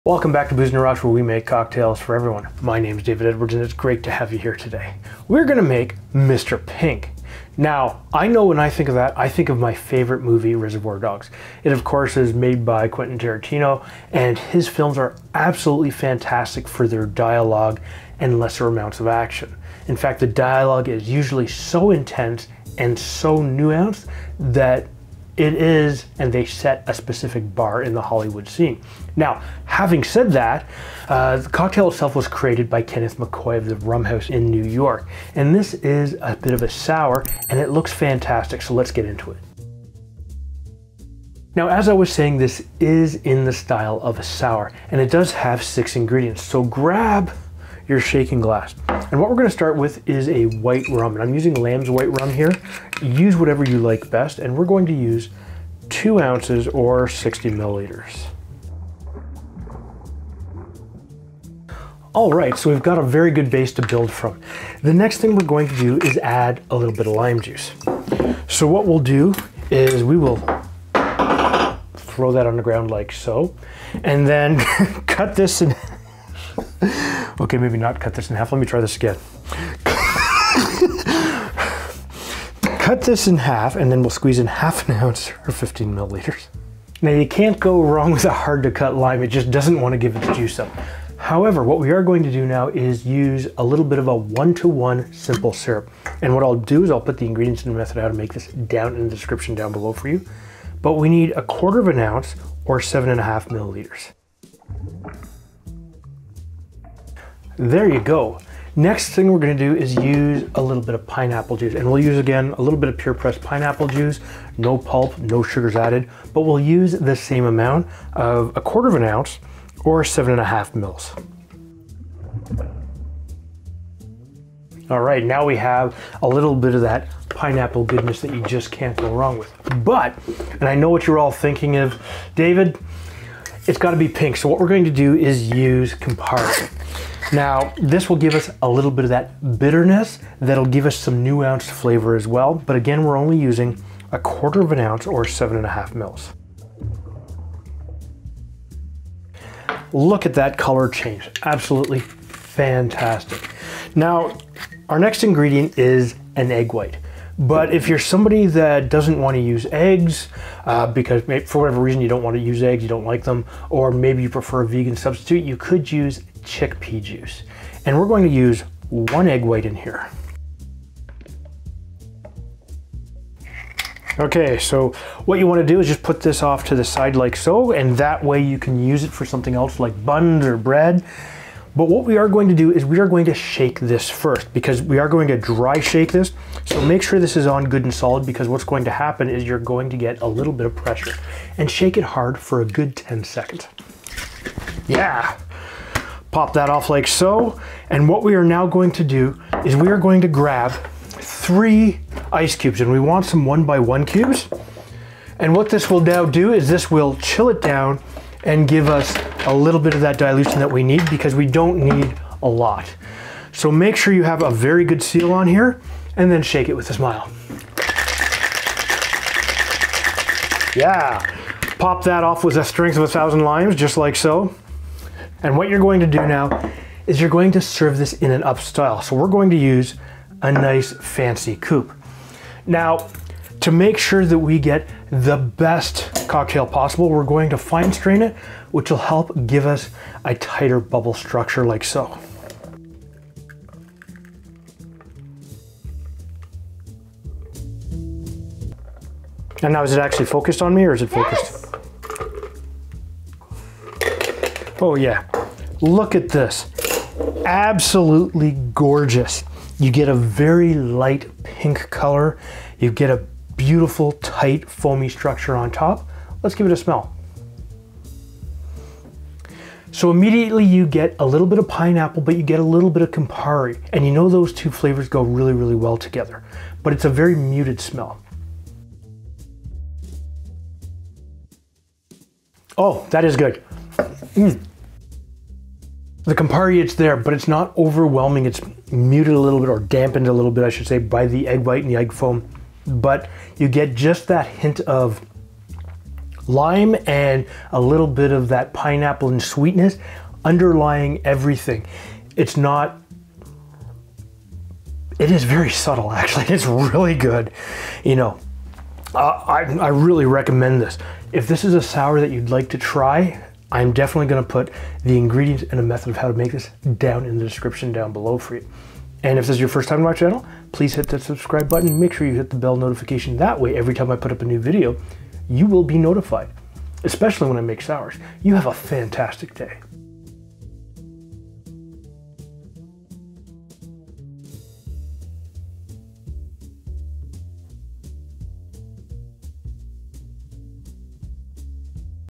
How To Make The Mr. Pink Cocktail | Booze On The Rocks
Today on Booze On The Rocks we show you how to make the Mr Pink Cocktail. This cocktail was created by Kenneth McCoy from the Rum House bar in New York and is based on the hit movie Reservoir Dogs filmed by Quentin Tarantino.  Its subtle flavors blend Campari, pineapple juice and lime together to give you a gentle sour that is easy to drink.  Try it out and let me know what you think.

Chapters

Intro: 0:00
Make The MR PINK: 0:39
The Taste: 4:28
Sign Off: 5:18

BAR TOOLS

MR PINK

2 oz/60 ml White Rum
.5 oz/15 ml Lime Juice
.25 oz/7.5 ml Simple Syrup ***
.25 oz/7.5 ml Pineapple Juice
.25 oz/7.5 ml Campari
1 Egg White

Method

1. Add all ingredients to the shaker tin.
2. Shake without ice for 10 seconds.
3. Shake with ice for 10 seconds.
4. Pour into a coupe.

4 oz/120 ml Water
4 oz/120 g Sugar

Method

1. Add both ingredients to a pot on the stove.
2. Heat slowly and stir until the sugar is fully incorporated.
3. Allow to cool before use.